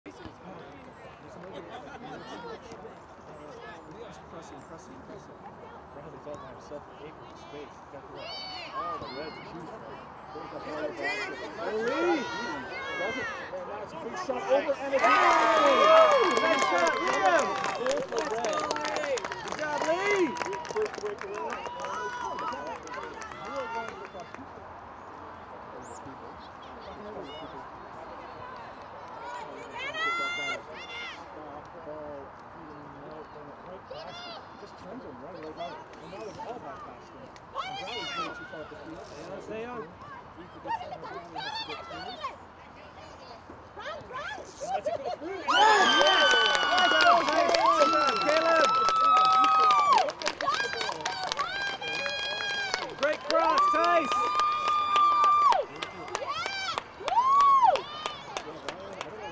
2023-10-14 B11 ECNL vs Crossfire 2 Goals
The goals from todays game against Crossfire ECNL 2.

Goal 1: Liam
Goal 2: Caleb from Tyce